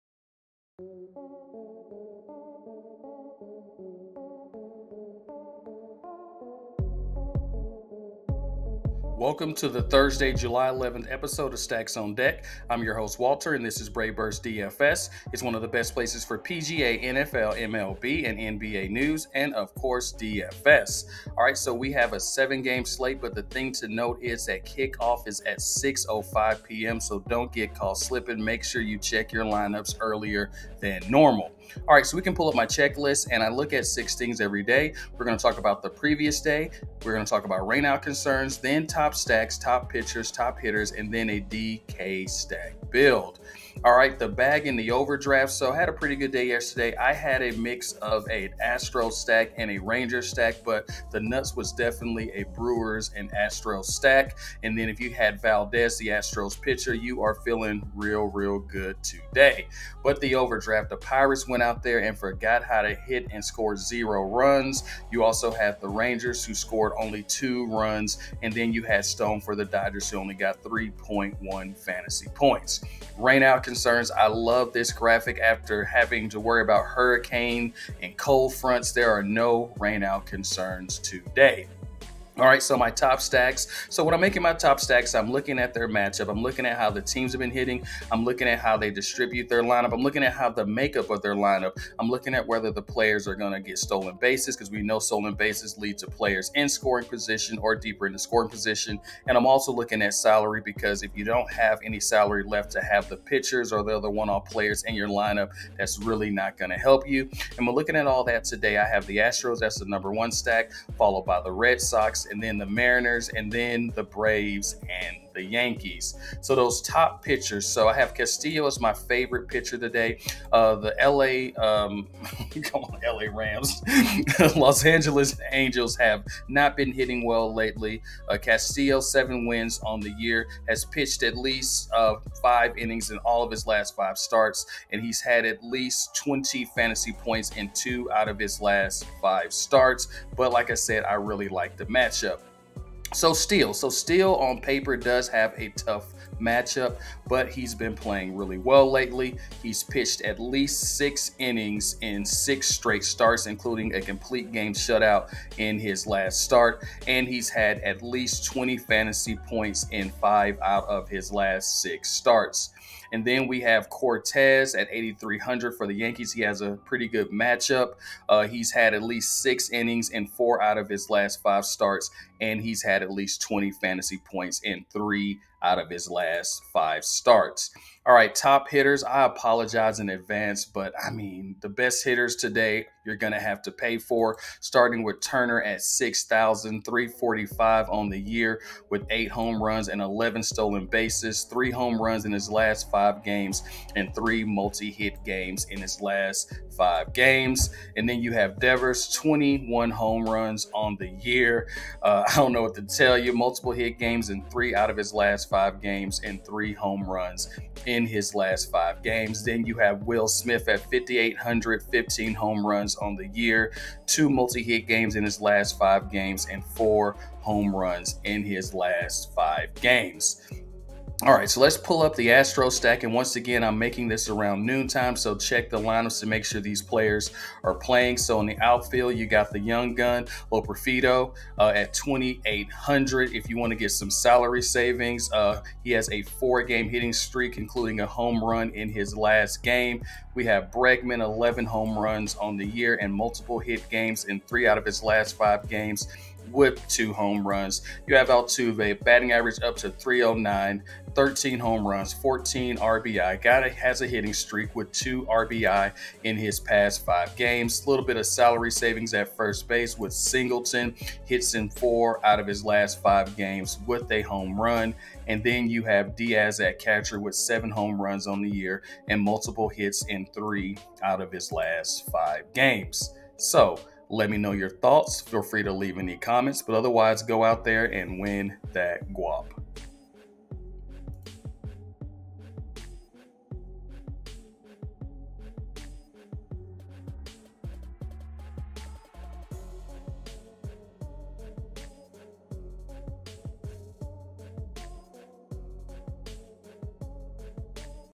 Thursday 7/11/2024 | MLB DFS Strategy | Draftkings | Advice | Lineup Help | Recommendations
Welcome to "Stacks on Deck" my DFS show where I give you an overview of the weather, best stacks, best hitters, and best pitchers to play everyday -- from a Draftkings DFS perspective.